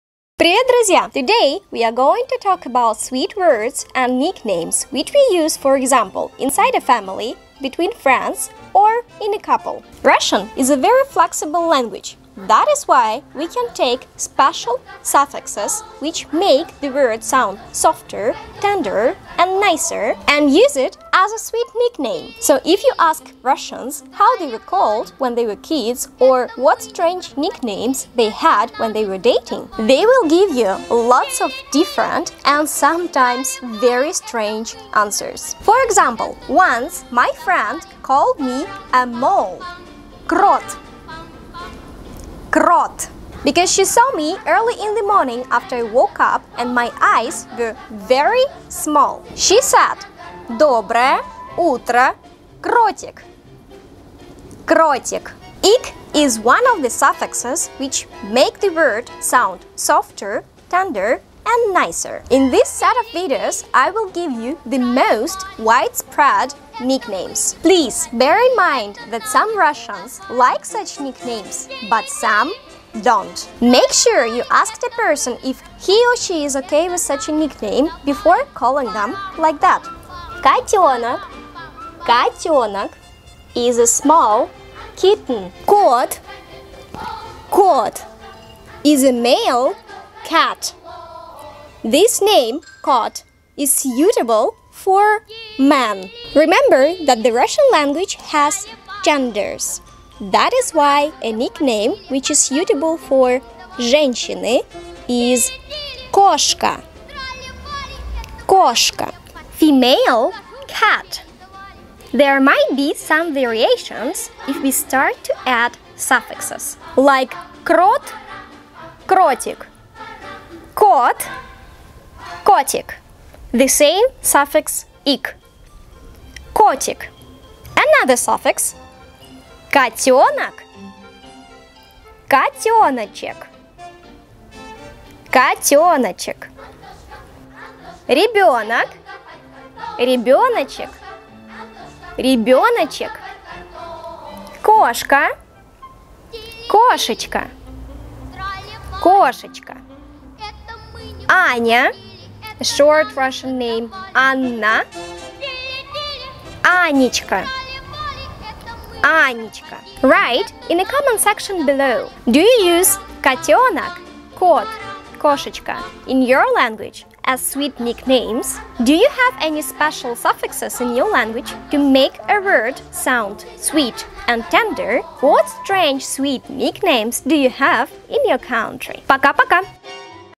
Sweet nicknames in Russian. Part 1 - КОТЁНОК
👩‍🎓 Words:
1️⃣ кот - male cat
2️⃣ кошка - female cat
3️⃣ котёнок - kitten
4️⃣ Доброе утро - Good morning!
🍀 Add suffixes!
1️⃣ крот - кротик
2️⃣ кот - котик
3️⃣ котёнок - котёночек
4️⃣ ребенок - ребёночек
5️⃣ кошка - кошечка
6️⃣ Аня - Анечка